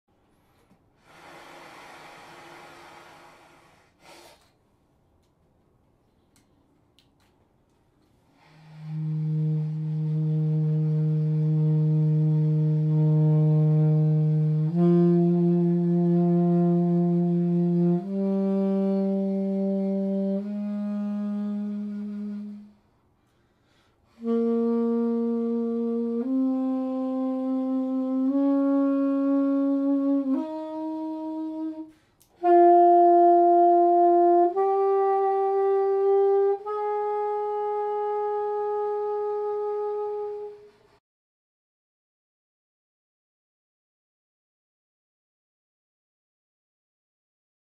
Smokehouse Saxophone 1
This title is so literal I can't stand it.

I intentionally smoked out this saxophone and unintentionally smoked up the entire floor in my apartment building.  Why was I compelled to do this?  I asked myself this question many times during the many takes.  I don't know.  Why does anybody bother with any artistic endeavor?  I guess it's an expression of something that is difficult to put into words.